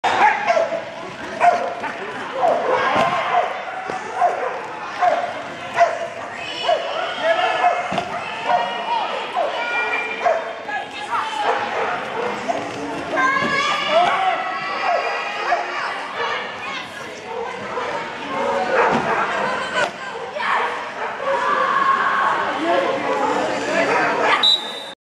MCR Spring Fling 2013 - Shut the Front Door, Race 147, Heat 1
4/14/13 - FM Shut the Front Door (Lucy, Breezie, Curly, and Karmela) running against WMM Chaos.